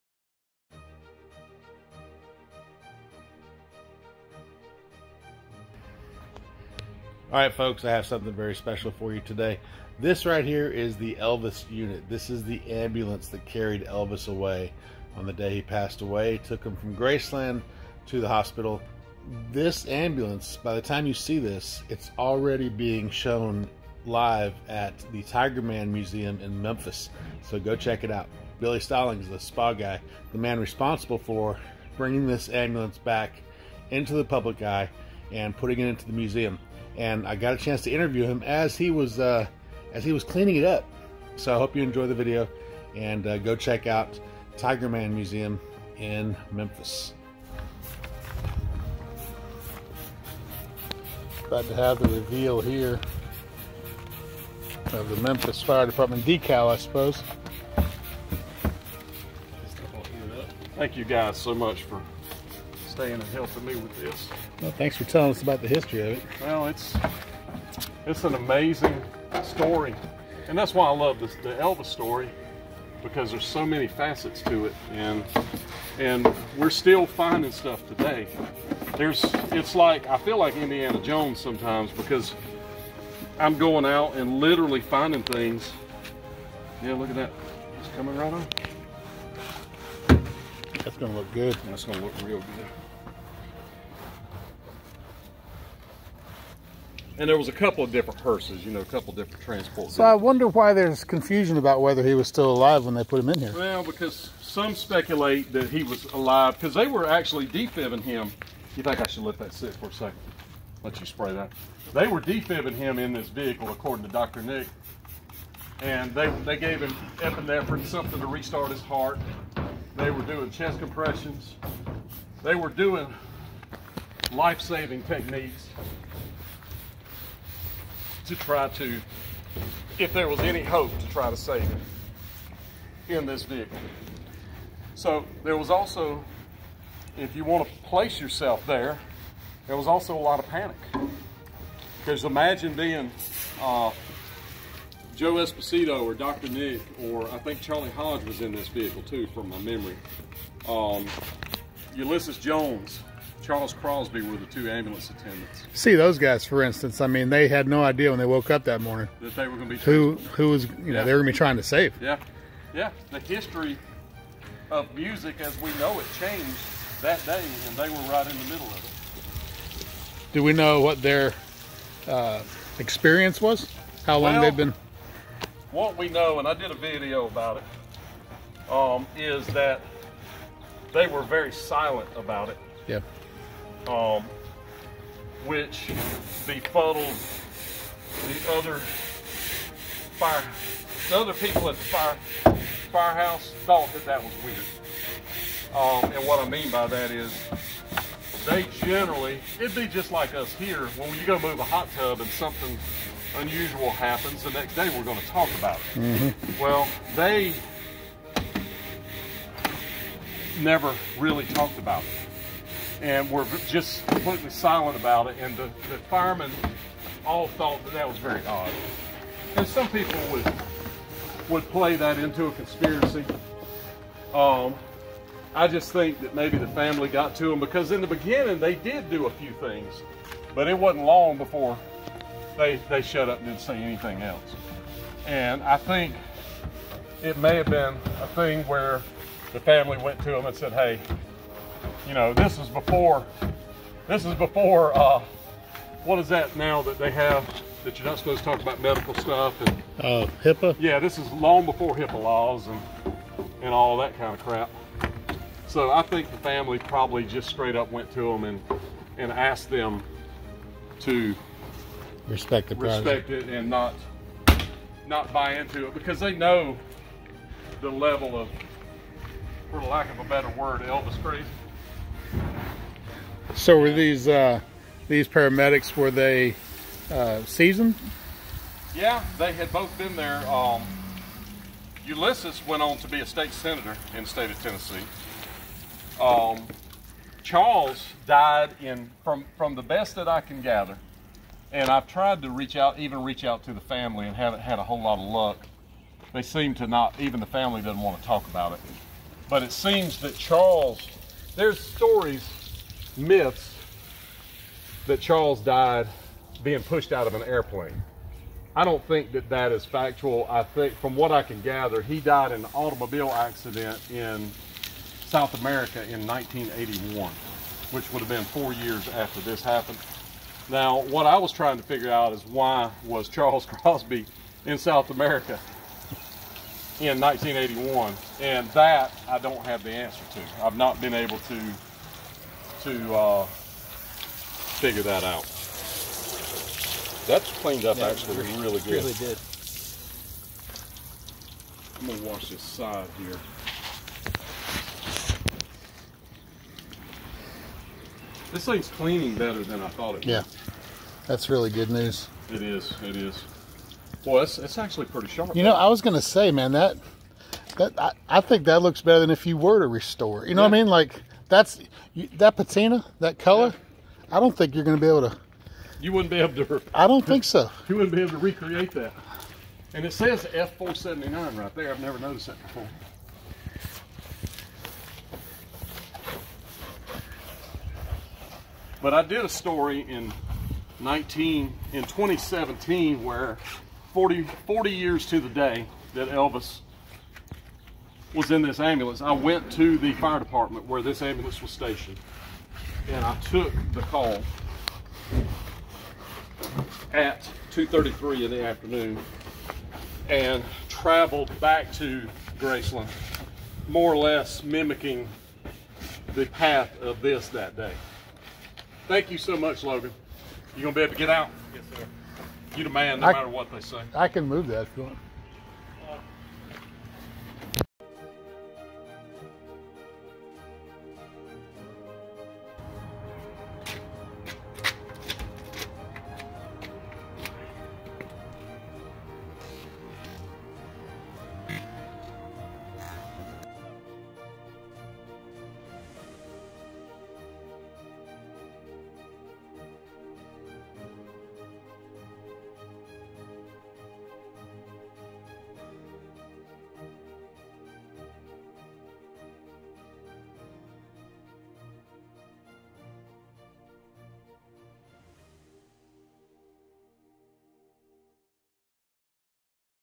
Elvis Presley Ambulance MFD PARAMEDICS "Cleaning Up History" Episode 6 Behind The Scenes Interview
In this episode the Spa Guy talks about the mystery, and conspiracies surrounding the paramedics who drove the ambulance to Graceland that fateful day.

This is a rare behind the scenes interview series with Elvis Presley historian and YouTuber Billy Stallings, who goes by "The Spa Guy." Missing for decades, Billy was able to find and acquire the ambulance that carried Elvis Presley from Graceland to Baptist hospital the day he died. Before placing the ambulance in a museum, The Spa Guy cleaned it up. While cleaning it, he agreed to be interviewed. Normally Stallings is telling stories and asking questions from behind the camera. But for these episodes, he is the person of interest.